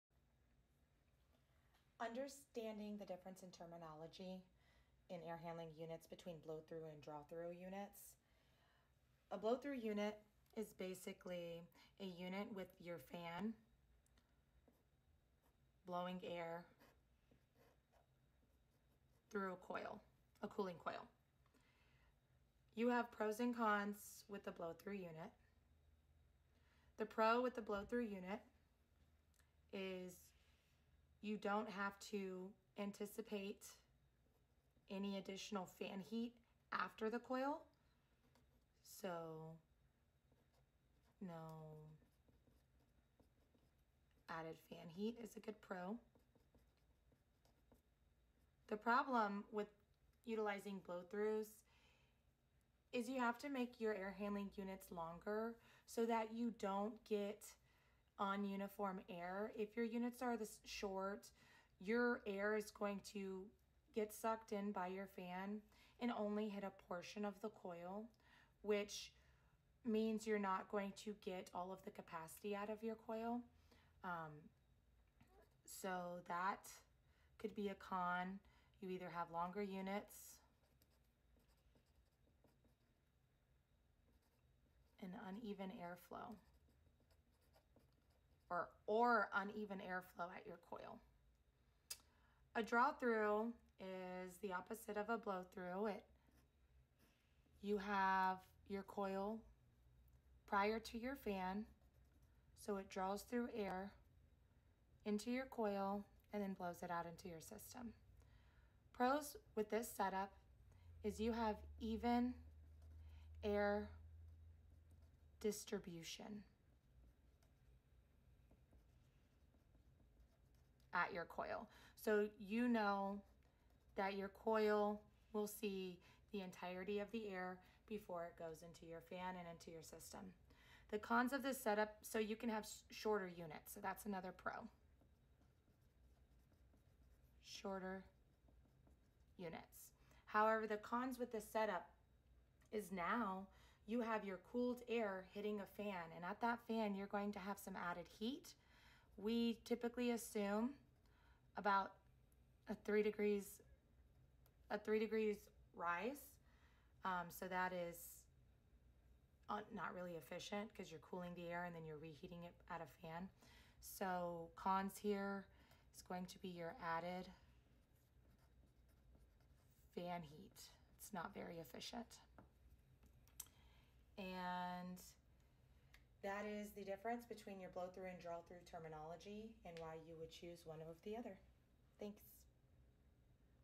Understanding the difference between a draw thru and blow thru unit
@hvacengineeringdesign air handling unit AHU blow thru vs draw thru considerations.